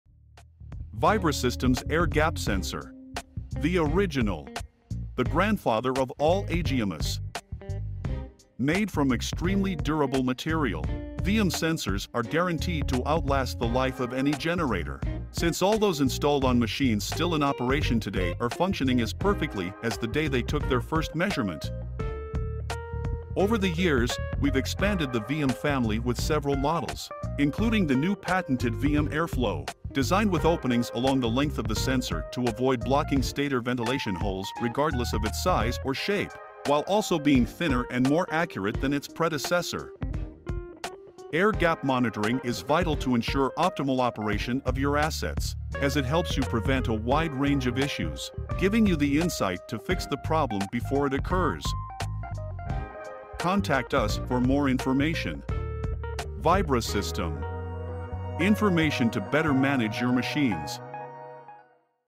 Prevent Multiple Issues With Air Gap Sensors
VibroSystM's air gap sensor. The original. The grandfather of all AGMS. The VM family has several models, including the new patented VM airflow, designed with openings along the length of the sensor to avoid blocking stator ventilation holes regardless of its size or shape (U.S. Patent No. 11125795). Air gap monitoring helps you prevent a wide range of issues, giving you the insight to fix the problem before it occurs.

Get all the latest information and technical tips to better manage your assets by visiting us on the following platforms. 👇